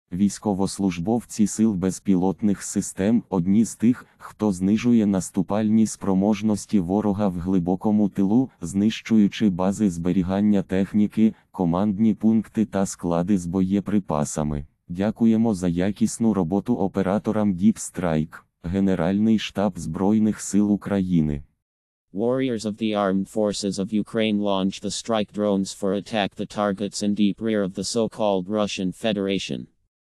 Зброя для ударів по глибокому тилу так званої рф
"Військовослужбовці Сил безпілотних систем - одні з тих, хто знижує наступальні спроможності ворога в глибокому тилу, знищуючи бази зберігання техніки, командні пункти та склади з боєприпасами.

Дякуємо за якісну роботу операторам “deep strike”! ",

Warriors of the Armed Forces of Ukraine launch the strike drones for attack the targets in deep rear of the so-called russian federation

Глибоко і боляче — Сили оборони показали зброю для ударів по далекому тилу рф – АрміяInform

Запуск “deep strike”. | Генеральний штаб ЗСУ / General Staff of the Armed Forces of Ukraine

Дякуємо за якісну роботу операторам “deep strike”! - YouTube